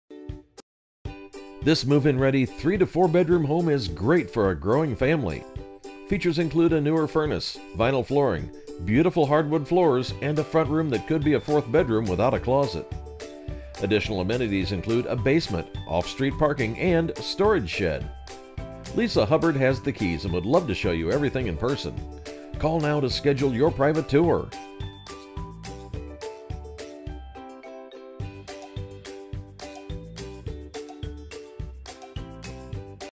910 Elm Street Shelbyville, IN | MLS# 21540589
Lisa Hubbard- |

Ready to move into 3-4 bedroom home. Great for the growing family. Newer furnace, some windows, vinyl flooring. Beautiful hardwood floors. Front room could be 4th bedroom without a closet.  Laundry room. Appliances stay. Basement. Off street parking. Storage shed. Nice sized fenced back yard.